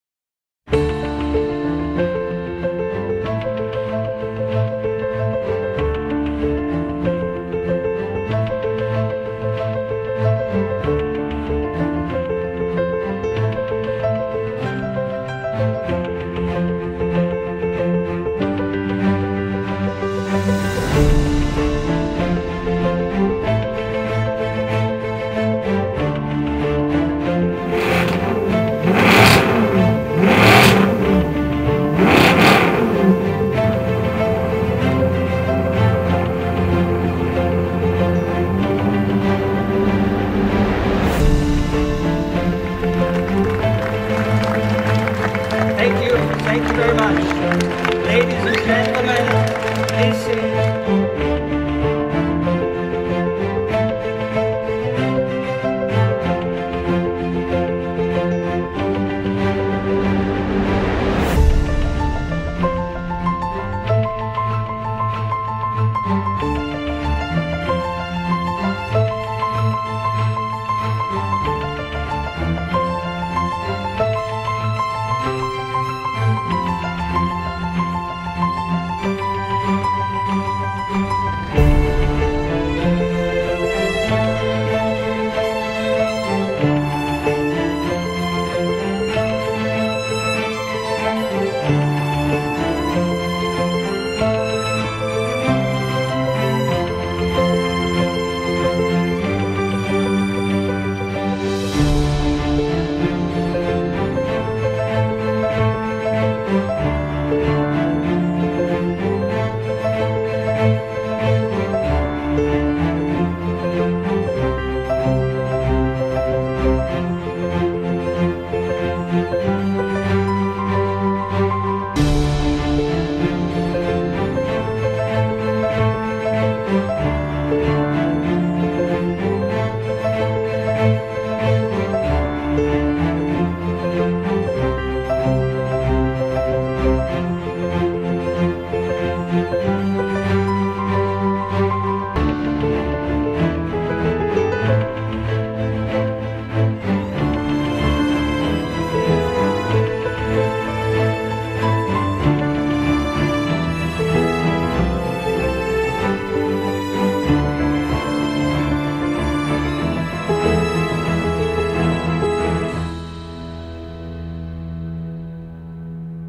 Maserati at Geneva Motor Show 2014
Maserati chose 2014 Geneva International Motor Show for the world debut of the Alfieri, a new concept that explores Maserati's stylistic heritage and hints at the brand's future design language.
The Maserati Quattroporte Zegna Limited Edition, also on display at 2014 SIAG in its definitive form, was celebrated in an exclusive party inside Ermenegildo Zegna's Geneva Store.